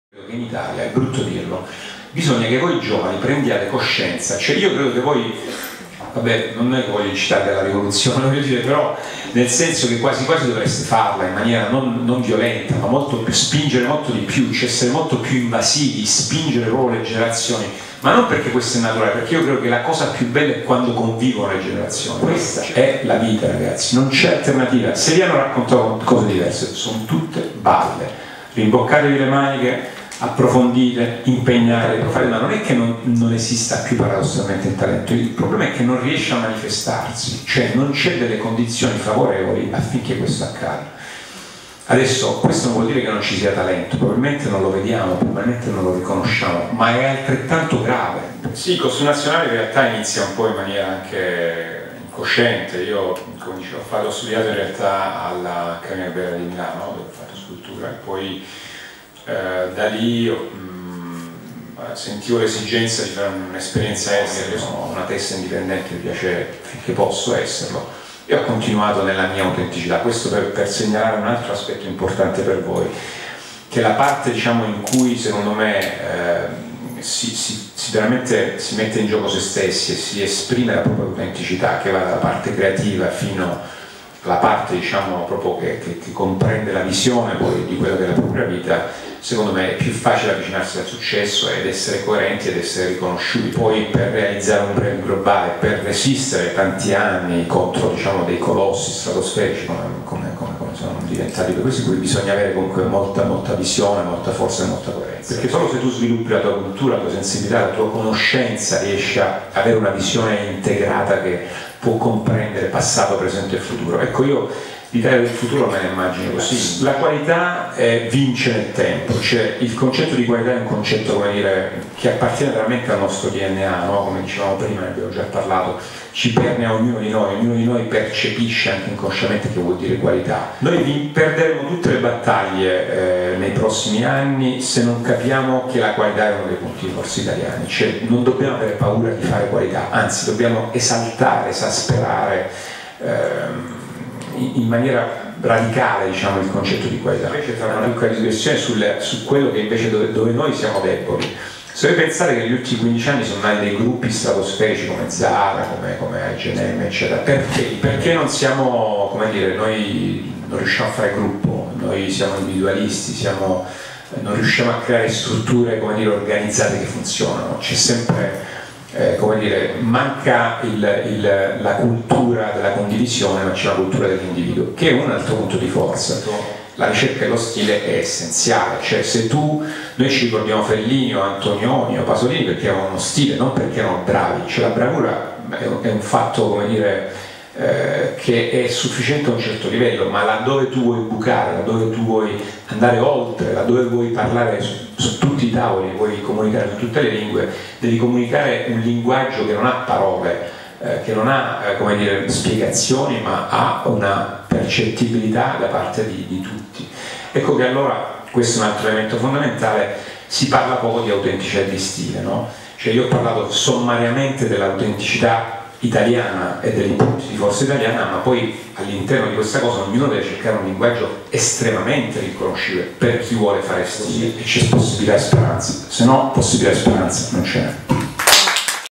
Capasa: Forza di volontà, indipendenza, determinazione. Le chiavi del successo.
(Video a cura di Mavi Caracuta)

"Bisogna essere perseveranti per ottenere risultati": così ha esordito Ennio Capasa, direttore creativo della maison Costume National, in occasione del decimo appuntamento del ciclo di seminari I Professionisti della Moda che lo ha visto protagonista giovedì 9 maggio all'Università La Sapienza di Roma. Durante l'incontro, lo stilista ha ripercorso le tappe fondamentali che hanno portato l'azienda, nata dal titolo di un vecchio libro francese sulle divise militari ritrovato in un mercatino, ad essere collocata, dopo 27 anni, tra i top player della moda.
Fortemente convinto che un'adeguata formazione, affiancata da un'esperienza internazionale, possa dare ai giovani designer la possibilità di sviluppare una conoscenza di passato, presente e futuro, lo stilista continua affermando che "la padronanza della materia prima è fondamentale nella percezione di uno stile"  e che "risulta essenziale ricercare uno stile proprio per poter emergere". Concetto molto importante in un contesto, come quello attuale, in cui intenet ha eliminato i confini conformando gli stili.
"Forza di volontà, indipendenza, determinazione, impegno" sono questi i punti di forza su cui devono lavorare le giovani "leve" della moda per cercare di comunicare un linguaggio che abbia la percepibilità da parte di tutti, ha concluso Ennio Capasa.

Antonella Caporaso